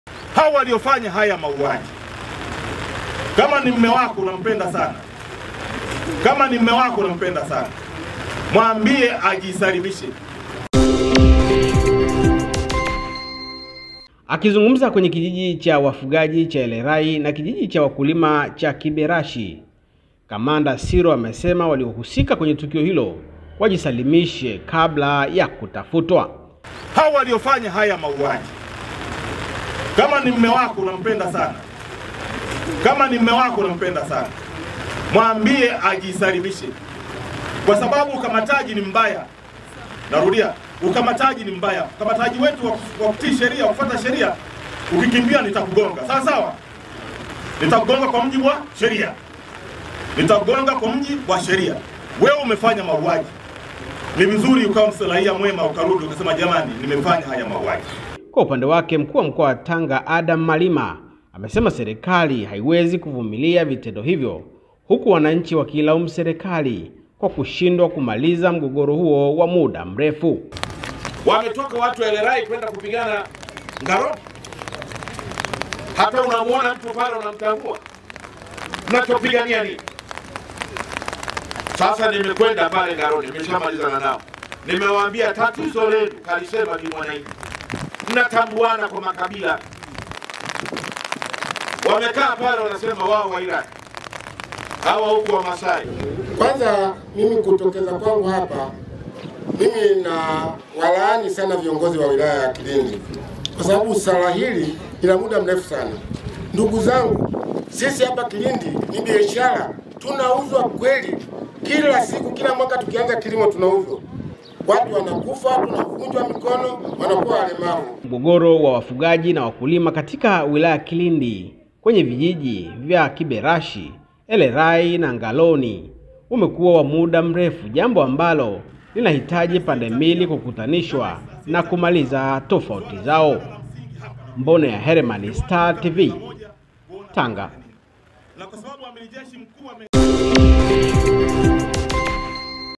MAUAJI YAENDELEA KULIGUBIKA TAIFA, WATU SITA TANGA WAUAWA KATIKA MAPIGANO, IGP ATOA MSIMAMO WAKE.
Mkuu wa jeshi la polisi Nchini IGP Simon Sirro amewataka walioshiriki kwenye mauaji ya watu sita kwenye mapigano kati wafugaji na wakulima wa kijiji cha Kibirashi, Elerai na Ngaloni wilayani Kilindi kujisalimisha mara moja.
Mpaka sasa watu takriban 20 wanashikiliwa na polisi wakihusishwa na mauaji hayo.